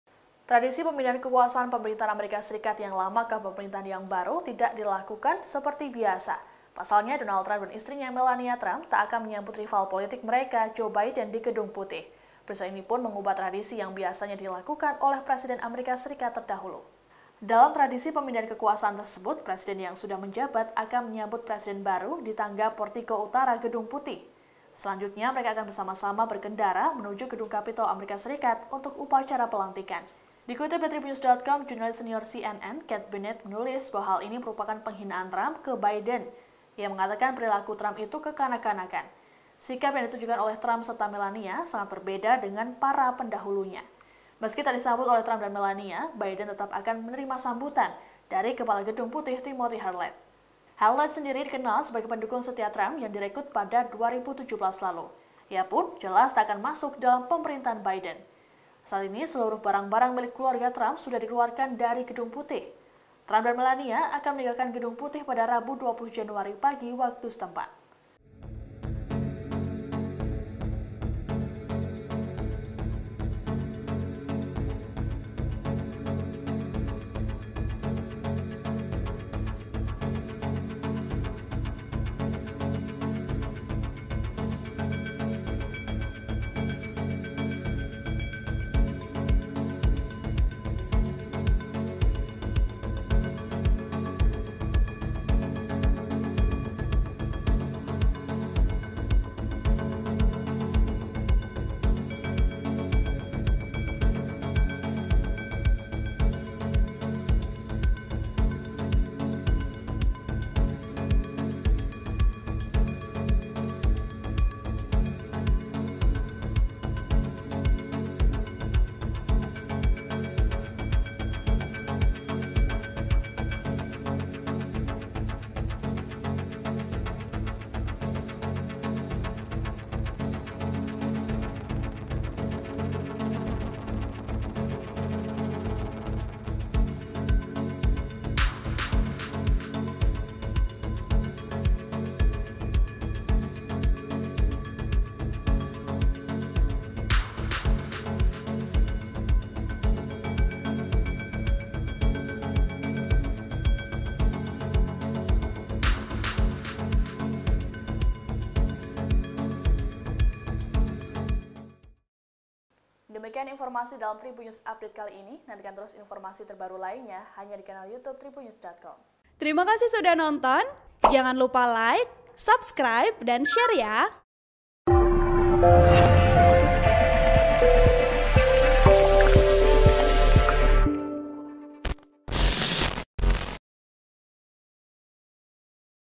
Trump dan Melania Tak Sambut Biden dan Istri di Gedung Putih, Ubah Tradisi Pemindahan Pemerintahan
TRIBUN-VIDEO.COM - Tradisi pemindahan kekuasaan pemerintahan Amerika Serikat (AS) yang lama ke pemerintahan baru tak dilakukan seperti biasa.

Pasalnya, Donald Trump dan istrinya, Melania Trump tak akan menyambut rival politik mereka, Joe Biden di Gedung Putih.

Peristiwa ini mengubah tradisi yang biasanya dilakukan oleh Presiden AS terdahulu.

Dalam tradisi pemindahan kekuasaan tersebut, presiden yang sudah menjabat akan menyambut presiden baru di tangga Portico Utara Gedung Putih.

Selanjutnya, mereka akan bersama-sama berkendara menuju Gedung Cpaitol AS untuk upacara pelantikan.

Dikutip dari Tribunnews.com, jurnalis senior CNN, Kate Bennet menulis, bahwa hal ini merupakan penghinaan Trump ke Biden.

Ia mengatakan, perilaku Trump itu kekanak-kanakan.

Sikap yang ditunjukkan oleh rump serta Melania sangat berbeda dengan para pendahulunya.

Meski tak disambut oleh Trump dan Melania, Biden tetap akan menerima sambutan dari Kepala Gedung Putih Timothy Harleth.

Harleth sendiri dikenal sebagai pendukung setia Trump yang direkrut pada 2017 lalu.

Ia pun jelas tak akan masuk dalam pemerintahan Biden.

Saat ini seluruh barang-barang milik keluarga Trump sudah dikeluarkan dari Gedung Putih.

Trump dan Melania akan meninggalkan Gedung Putih pada Rabu (20/1/2021) pagi waktu setempat.

(TribunVideo.com)